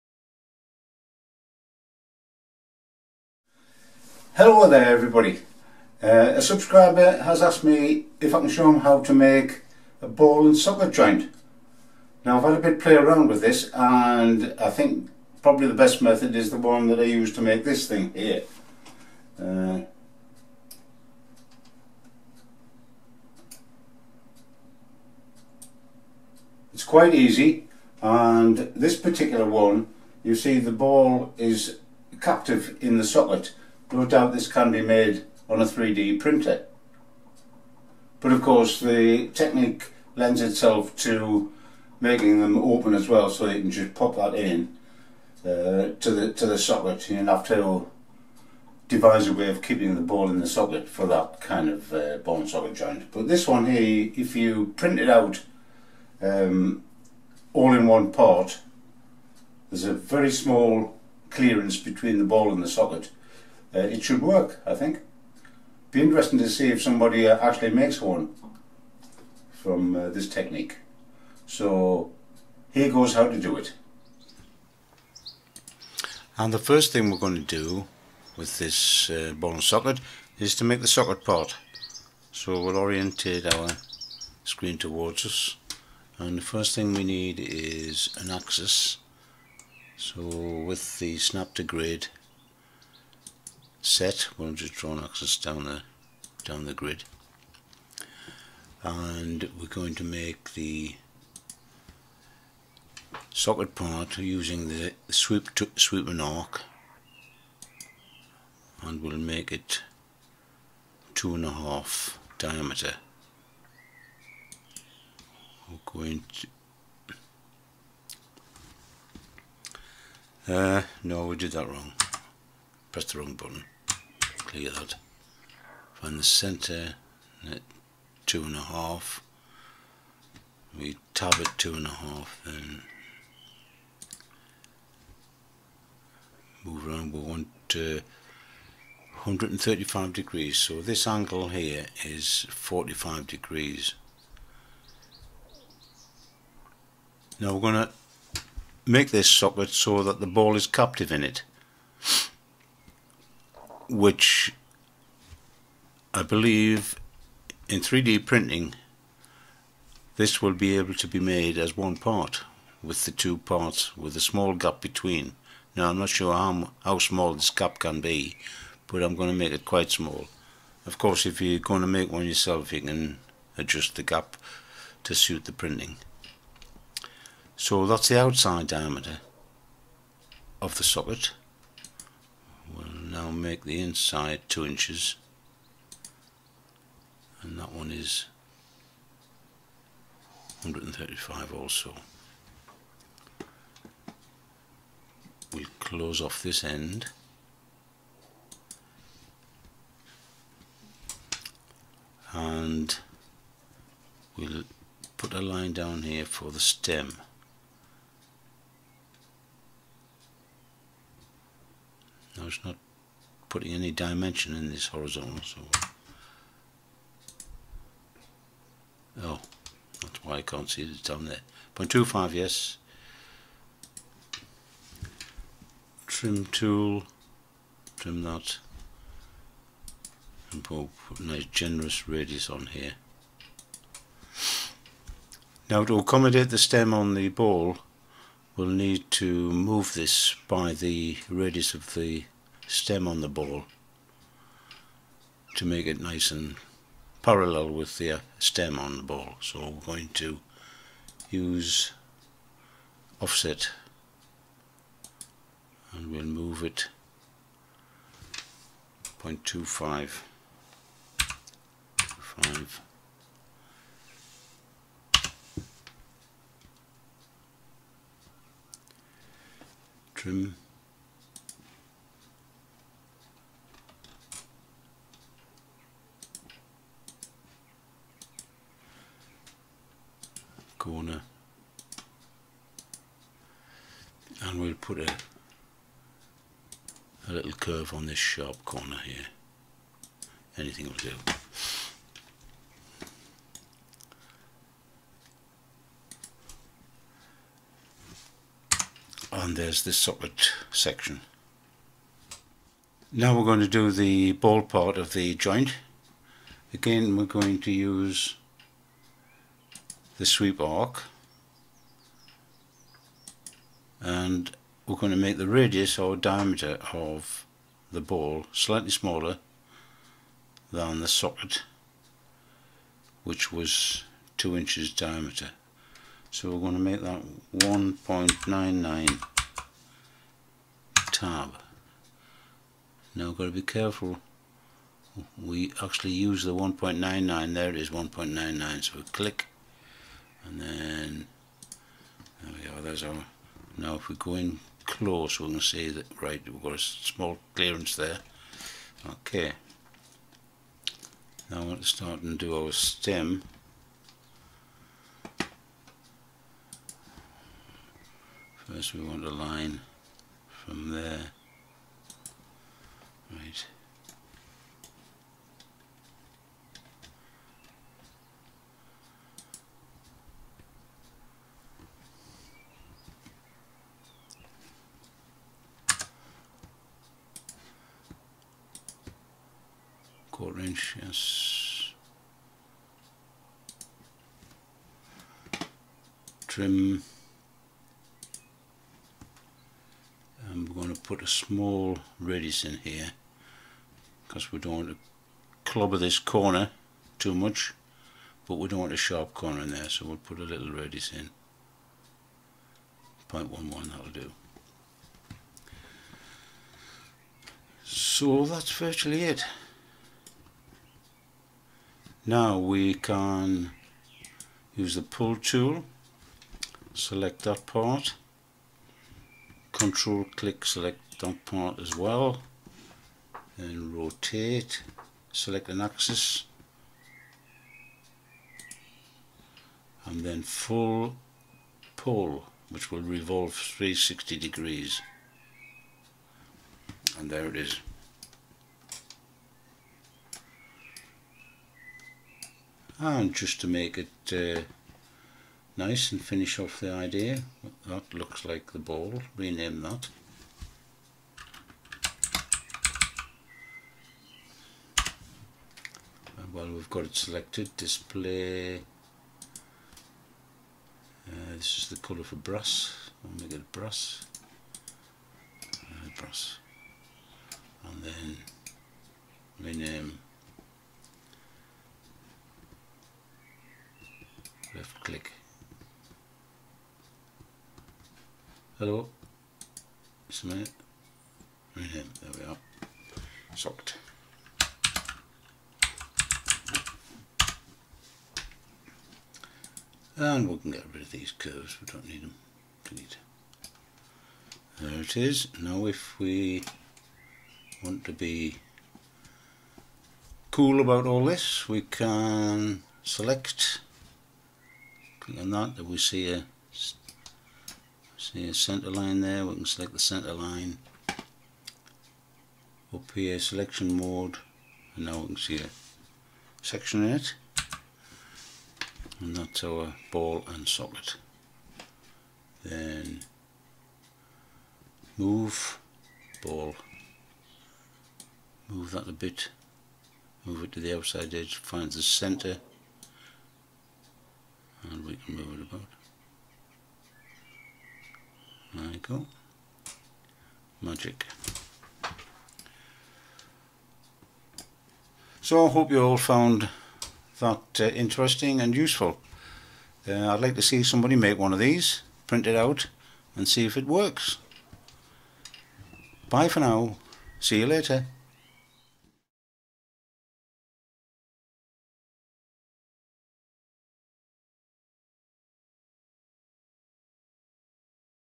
Making a ball & socket joint in DesignSpark Mechanical
If feel that you have benefited from this tutorial please consider donating 1GBP into my Paypal account - Bespoke videos can be requested via my A subscriber asked if I could show how to make one of these things and after trying a few ideas this seemed to be the easiest way to do it.